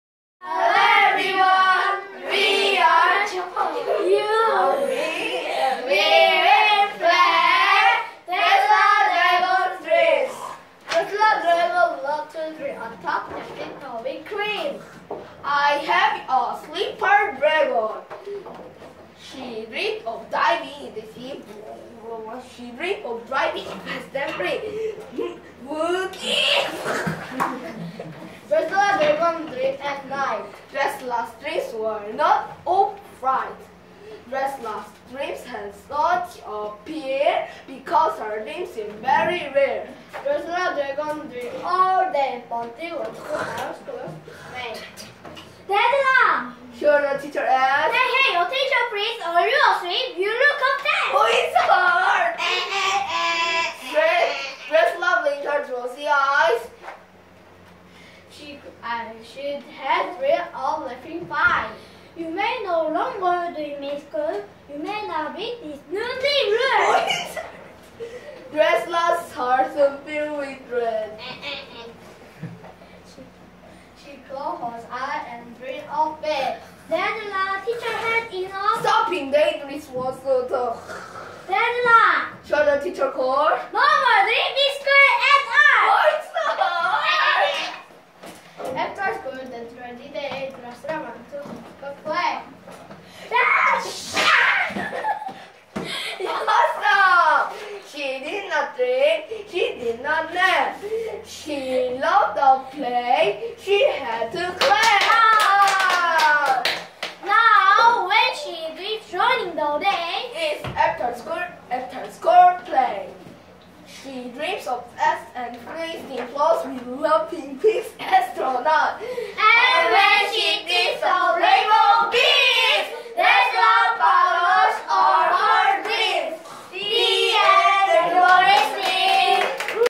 차일드유 월드컵 161 EC 3 Geo Drisella Dragon's Dream 박수민B 윤재성 조동현
차일드유 월드컵 캠퍼스 161회 (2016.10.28, 금요일) 영어발표회
(영어연극, 동화구연, 주제발표, 영어회화, 팝송동요 )